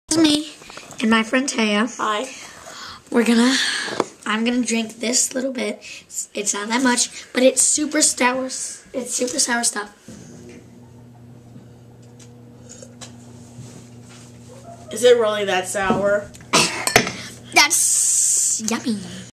devour the sour
Me drinking sour juice